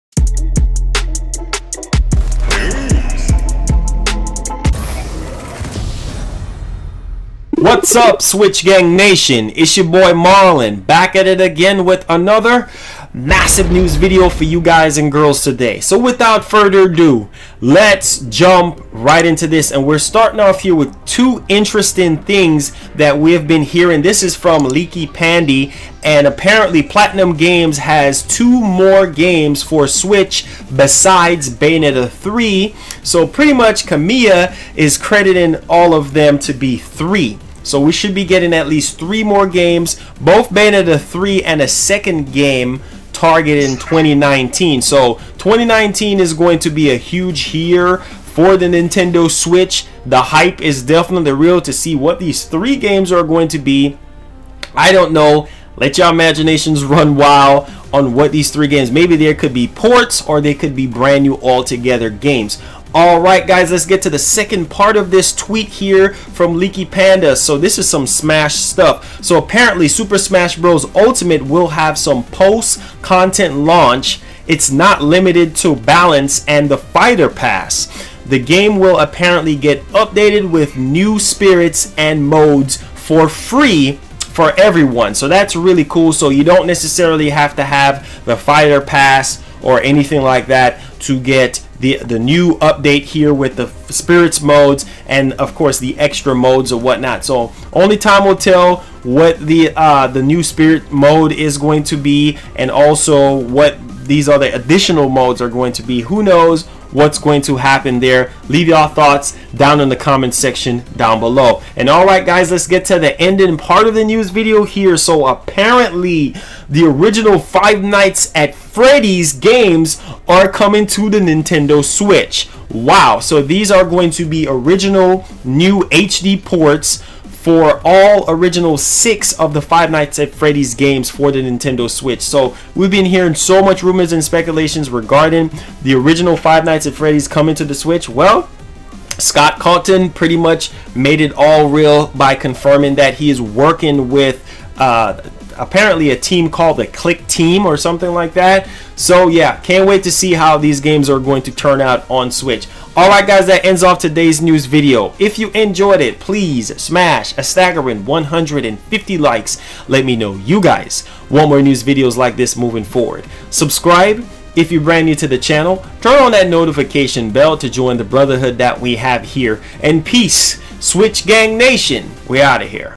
3 New Switch Platinum Games | Smash Ultimate Free DLC New Spirits & Modes (Rumor) & FNAF HD Switch
3 New Nintendo Switch Platinum Games  | Smash Ultimate Free DLC  New Spirits & Modes (Rumor) FNAF HD Switch

THE ORIGINAL FIVE NIGHTS AT FREDDY’S GAMES ARE COMING TO SWITCH